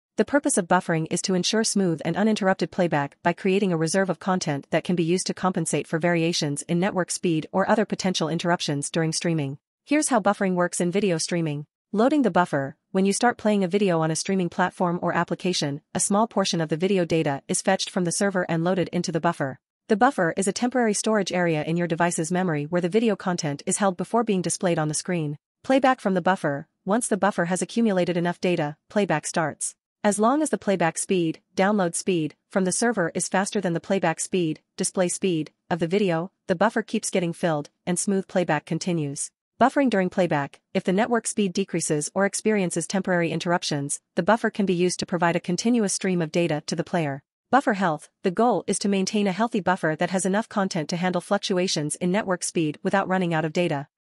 What is Buffering and how Buffering works?
Buffering, in the context of video streaming or multimedia playback, refers to the process of temporarily storing and loading a certain amount of video or audio data before playback begins. The purpose of buffering is to ensure smooth and uninterrupted playback by creating a reserve of content that can be used to compensate for variations in network speed or other potential interruptions during streaming.

Here's how buffering works in video streaming:

Loading the Buffer: When you start playing a video on a streaming platform or application, a small portion of the video data is fetched from the server and loaded into the buffer. The buffer is a temporary storage area in your device's memory where the video content is held before being displayed on the screen.

Playback from the Buffer: Once the buffer has accumulated enough data, playback starts. As long as the playback speed (download speed) from the server is faster than the playback speed (display speed) of the video, the buffer keeps getting filled, and smooth playback continues.

Buffering During Playback: If the network speed decreases or experiences temporary interruptions, the buffer can be used to provide a continuous stream of data to the player. This prevents interruptions and buffering delays during playback.

Buffer Health: The goal is to maintain a healthy buffer that has enough content to handle fluctuations in network speed without running out of data. A buffer that is too small might result in frequent pauses or stuttering during playback if there are network issues. On the other hand, an excessively large buffer may cause higher initial latency before playback begins.

Buffering is particularly important in streaming applications because it helps to handle variations in network quality, which can vary due to factors like internet congestion, server load, or the user's network connection type. By buffering content in advance, video streaming services can provide a smoother and more enjoyable user experience. However, excessive buffering can also be frustrating for users, so a balance between buffering enough data for smooth playback and minimizing initial delays is essential.